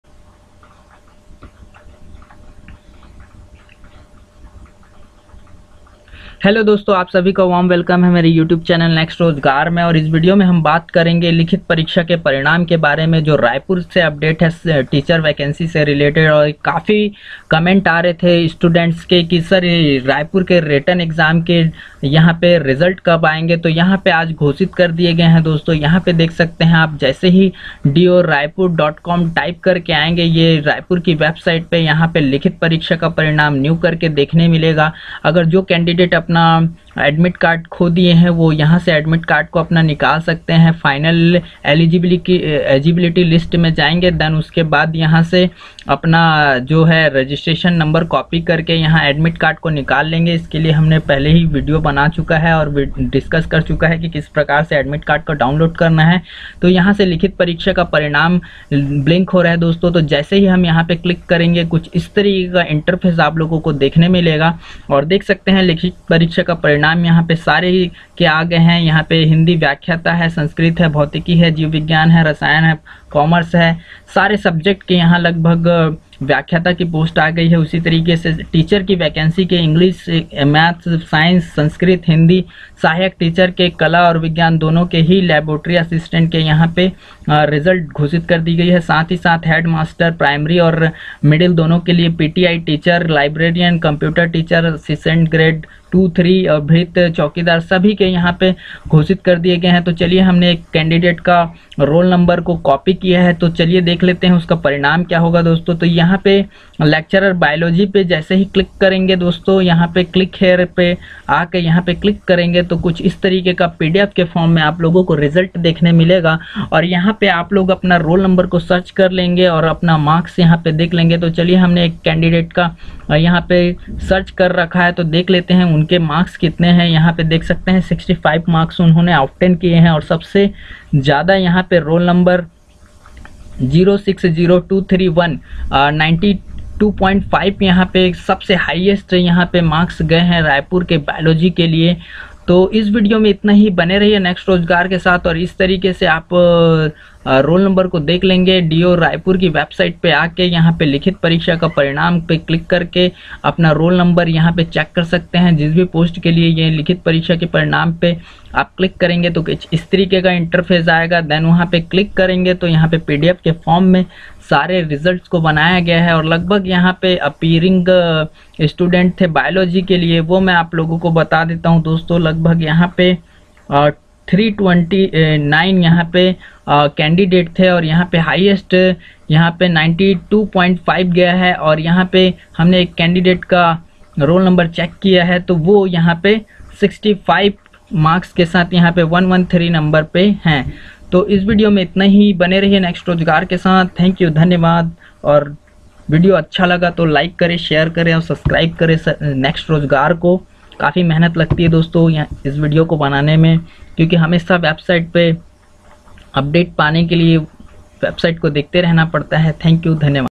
Raipur Written Exam Result 2021 || Raipur teacher vacancy 2021 || रायपुर लिखित परीक्षा परिणाम घोषित
नमस्कार दोस्तों  ! इस वीडियो में रायपुर स्वामीआत्मानंद छत्तीसगढ़ अंग्रेजी माध्यम स्कूल शिक्षक भर्ती 2021 रायपुर लिखित परीक्षा परिणाम  के बारे में बताया गया है।  इस वीडियो के माध्यम से हम आपको स्वामी आत्मानंद अंग्रेजी माध्यम विद्यालय रायपुर  के अंतर्गत जारी किये गए भर्ती विज्ञापन के सम्बन्ध में जानकारी साझा की गयी है।

Result dekhne ke liye  yha click kre sbi post k